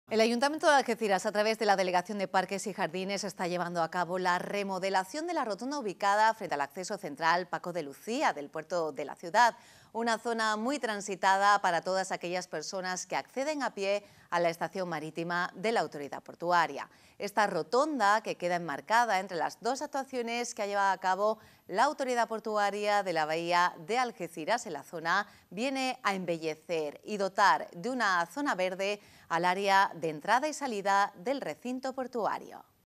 La colaboración entre Ayuntamiento y APBA permite mejorar la rotonda cercana al acceso central
El Ayuntamiento, a través de la Delegación de Parques y Jardines  está llevando a cabo la remodelación de la rotonda ubicada frente al Acceso Central “Paco de Lucía” del Puerto de Algeciras, una zona muy transitada para todas aquellas personas que acceden a pie a la estación marítima de la APBA.  Esta rotonda, que queda enmarcada entre las dos actuaciones que ha llevado a cabo la Autoridad Portuaria de la Bahía de Algeciras en la zona viene a embellecer y dotar de una zona verde al área de entrada y salida del  recinto portuario.